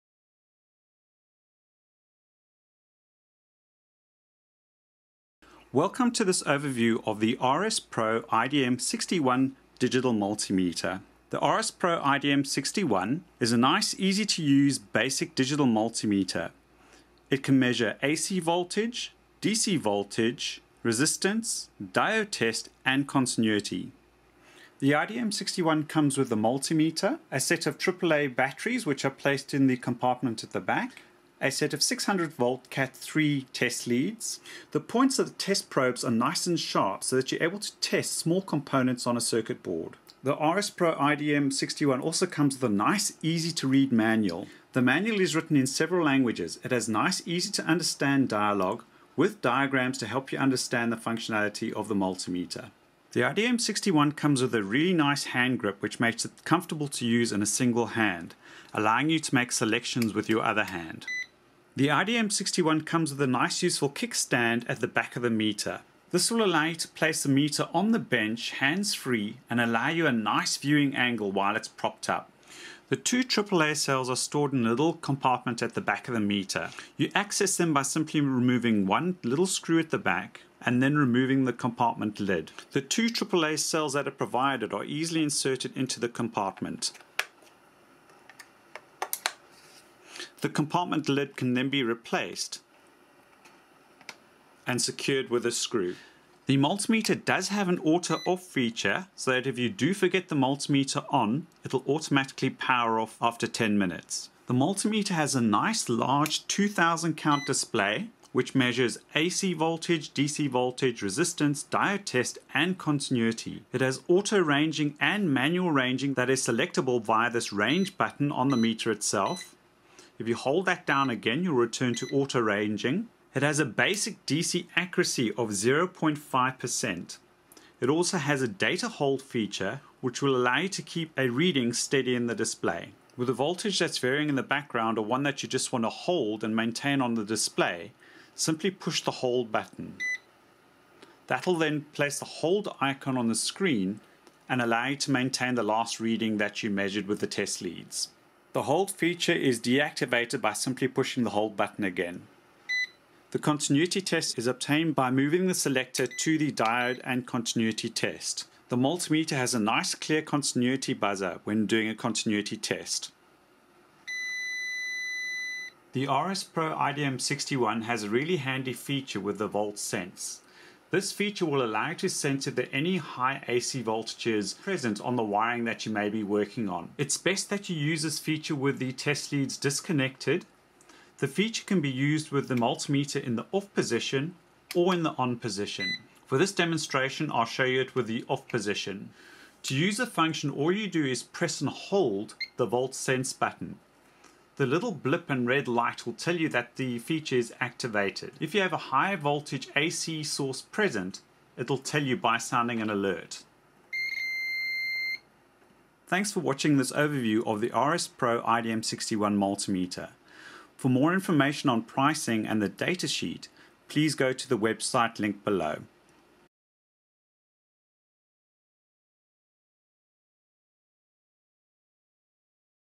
RS Pro IDM 61 Digital Multimeter Review
Martin Lorton reviews the RS Pro IDM 61 Digital Multimeter. The IDM 61 is a high performance pocket multimeter with a non-contact voltage detector. This multimeter measures AC voltage, DC voltage, resistance, diode test and continuity.